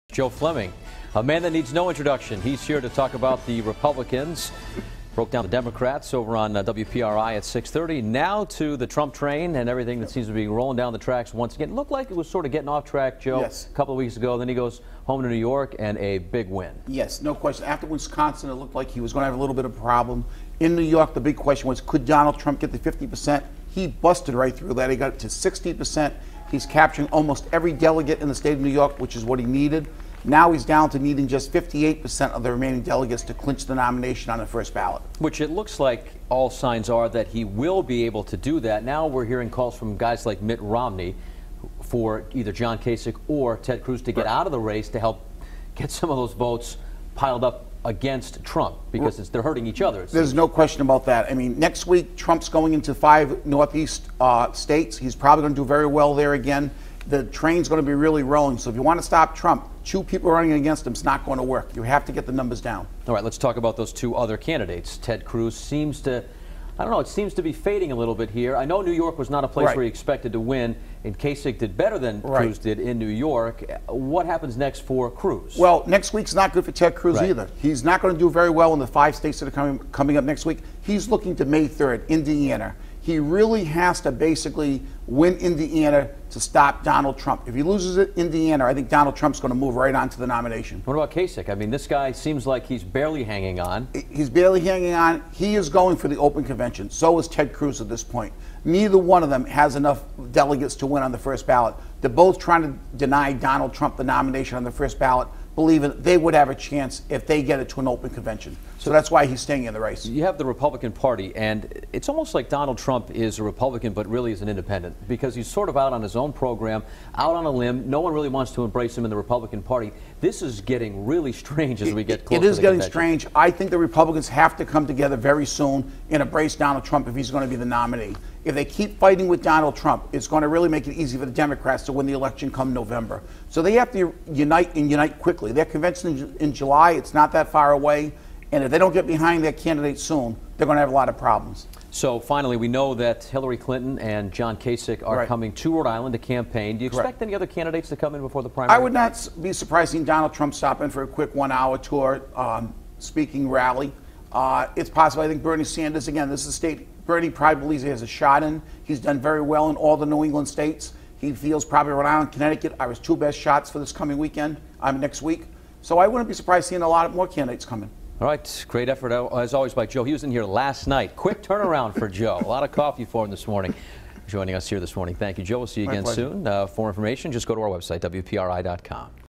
Analyst: Trump walks away with wins in delegate rich state
Joe Fleming joins Eyewitness News This Morning.